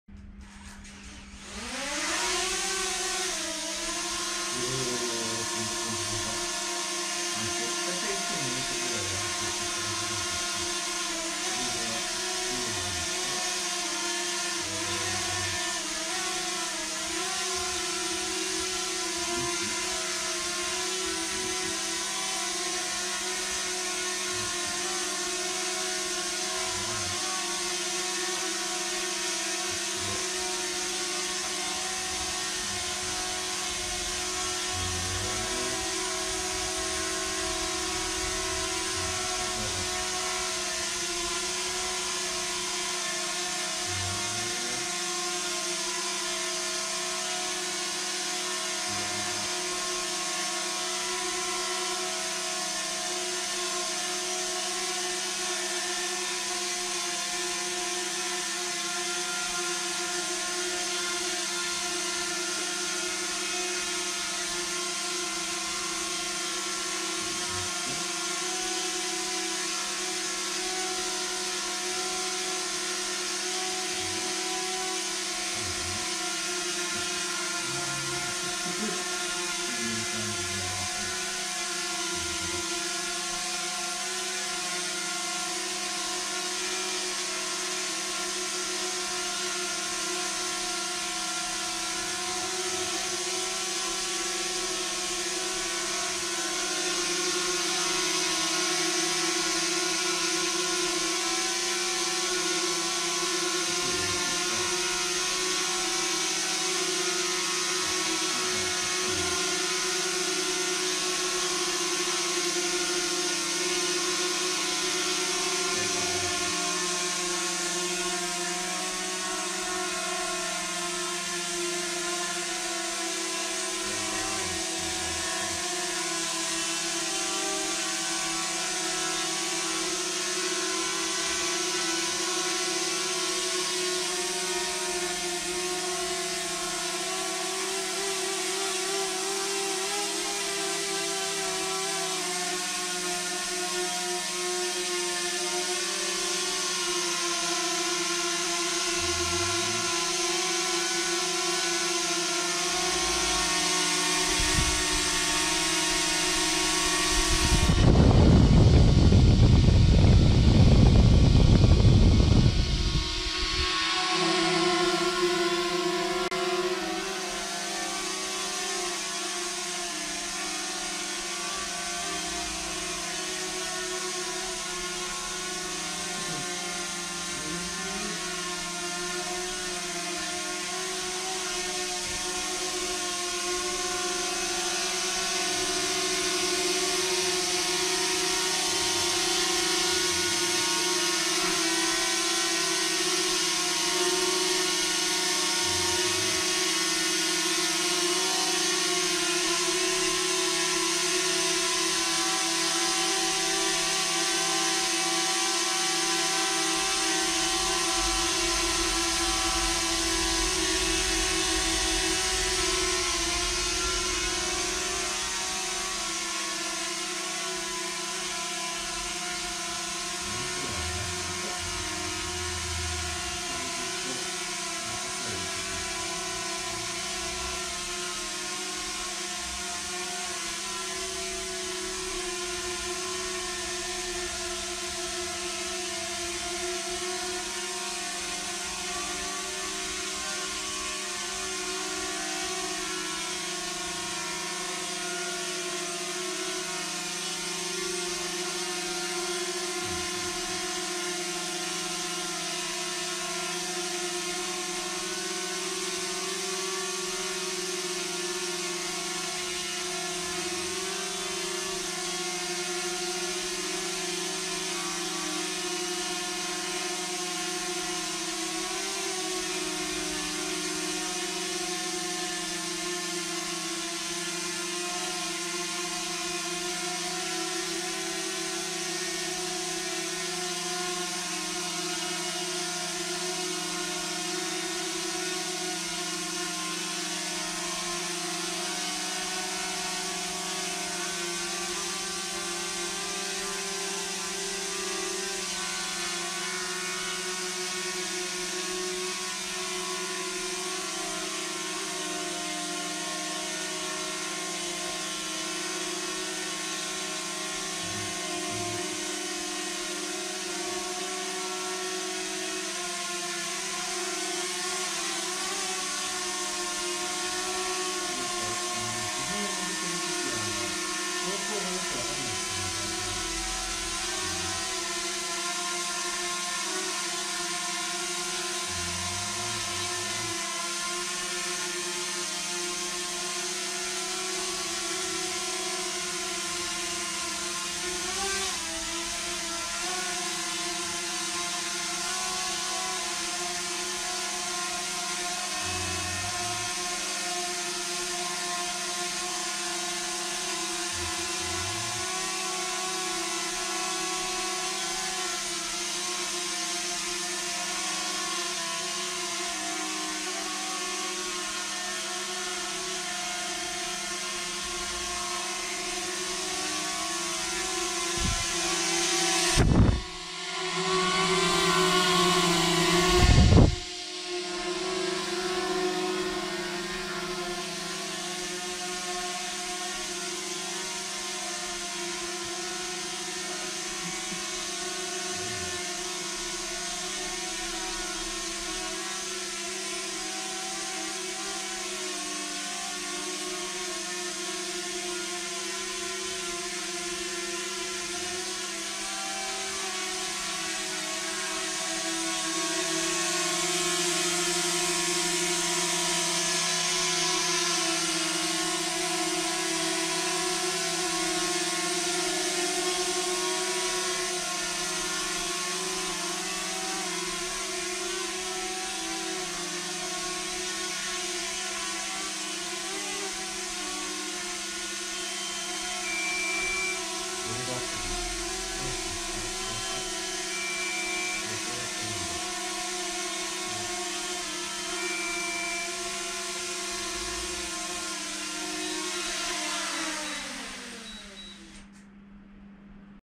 MAVIC MINI、初フライト
旅行の自撮り用に、小型ドローンMAVIC MINIを買ってみました。
今までPhantom 3という5年前のドローンを所有していましたが、とにかく重くて大きくて、出番はほとんどありませんでした。
MAVIC MINIは驚くほど小さく折り畳めるので、旅行バッグの隅に放り込んでおけば、200g以下の規制外の重量と相まって、使用する機会が多くなりそうです。
室内での初飛行は、壁や障子への衝突に備え、プロペラガードを付けて行いました。
屋内なのでGPSで制御できない飛行ですが、とても安定しているように思えました。